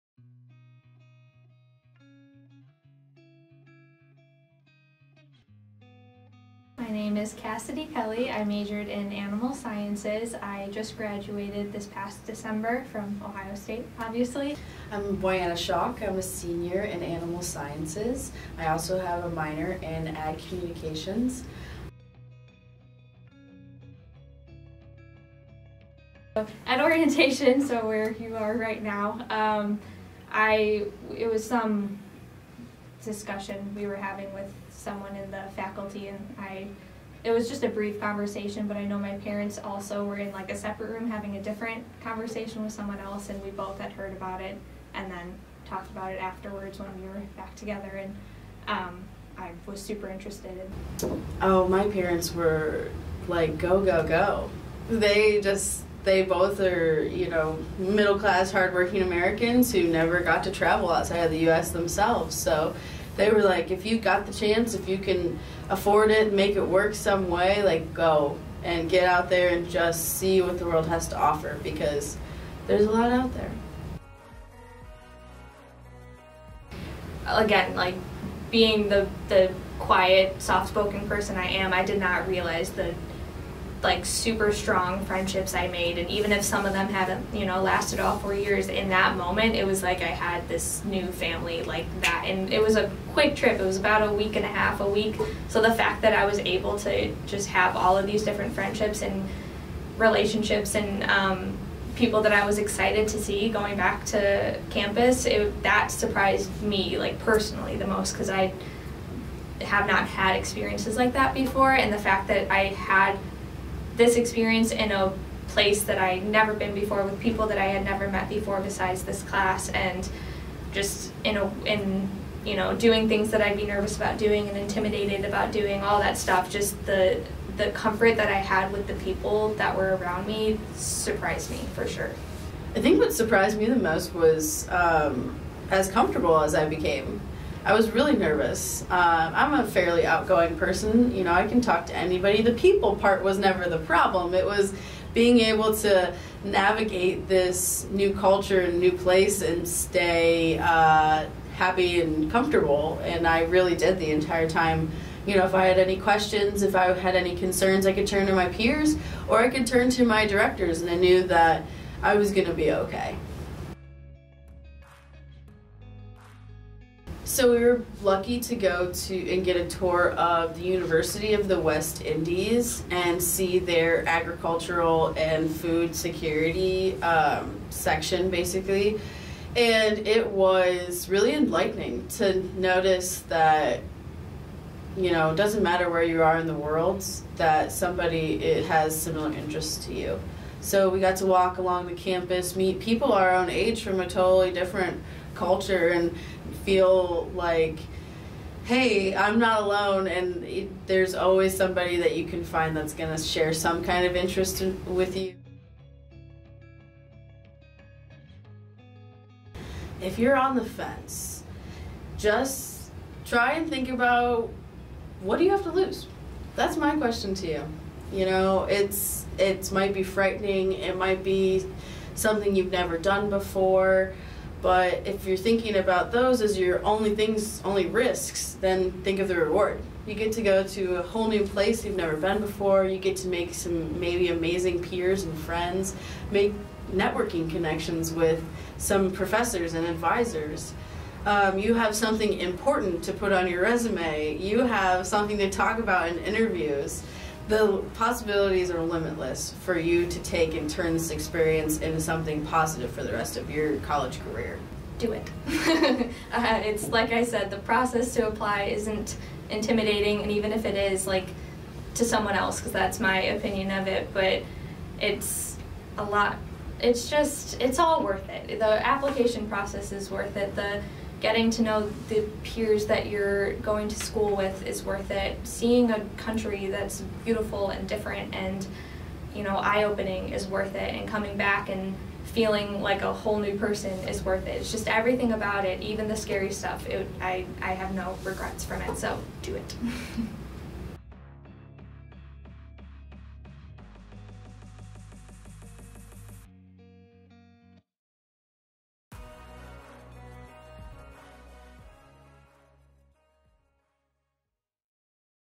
FAES First-year Student Experience - Past Participants
This amazing program gives CFAES students the opportunity to travel abroad in their first semester on campus as a Buckeye! Hear from past program participants on the value of the program in this video!

To learn more visit go.osu.edu/PeruFAES

The goal of this program is to provide an opportunity for students to learn about agricultural and environmental issues in an international context.

Planned visits during the program include food production, cultural and natural sites and animal health facilities in Peru. Presentations given by locals will range from economics to the cultural significance of the environment.

At the completion of this program, students will be able to successfully:

Compare and contrast agricultural issues, constraints and opportunities between Peru and the U.S.
Compare and contrast agricultural and environmental sustainability in Peru and the U.S.
Articulately discuss developmental and socioeconomic issues
Explore the impact of U.S. and global demands on Peru’s environment
Carry on a conversation about the role of agriculture in the global economy
Confidently travel to Peru and appreciate the culture